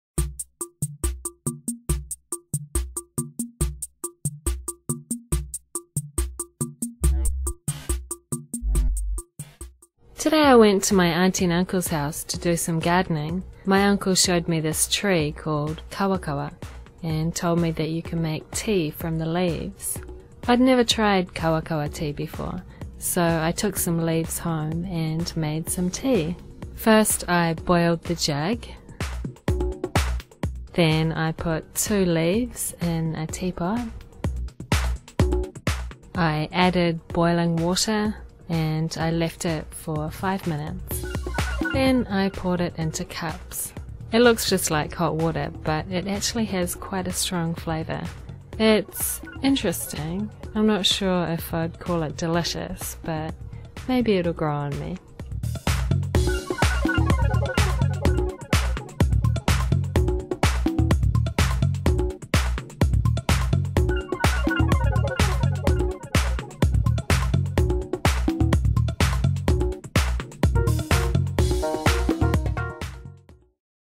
#1117 Kawakawa Tea
Sunday Kitchen #147  Kawakawa Tea

According to Wikipedia, this tree is only found in NZ ... so you'll have to come to New Zealand if you want to try this tea :)

Show 1117 Sunday 6 December
The Daily English Show